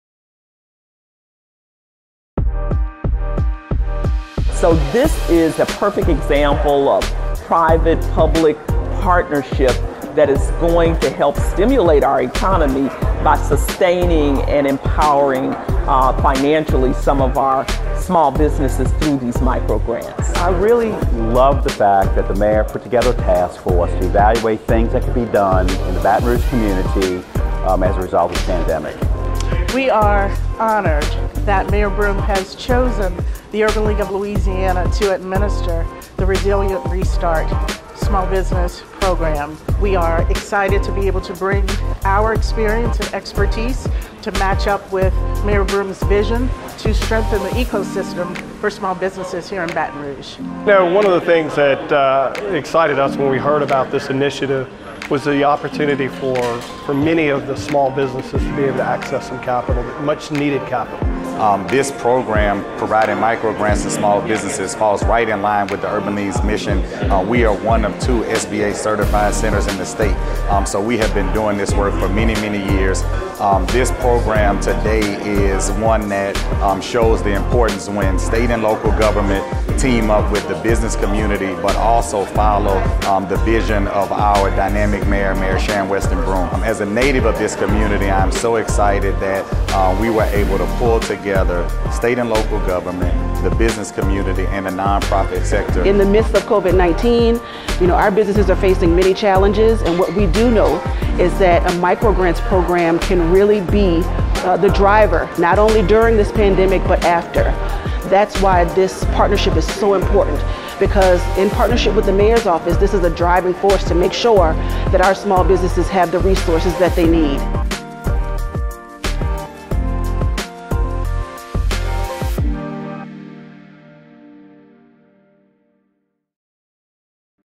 Resilient Restart Program
Have you applied for the Resilient Restart Program? This program was created to meet the short-term financial needs of micro-enterprises and small businesses, the Resilient Restart EBR: Small Business Micro-grants Program offers one-time, micro-grants of $2,500 to eligible entities that have been impacted by COVID-19.

Recipients may use these funds for rent, utilities, inventory, accounts payable, fixed costs, employee wages, and benefits. These are grant awards and repayment by the recipient is not required. Applications for micro-grants will be reviewed and awarded on a rolling basis until all allocated funds are dispersed.

This program is activated by the CARES Act administered through the Department of Housing and Urban Development’s Community Development Block Grant Program with additional funding contributions from Investar Bank, GMFS LLC, ExxonMobil Baton Rouge, and Postlethwaite & Netterville. This program is being administered by the Urban League of Louisiana.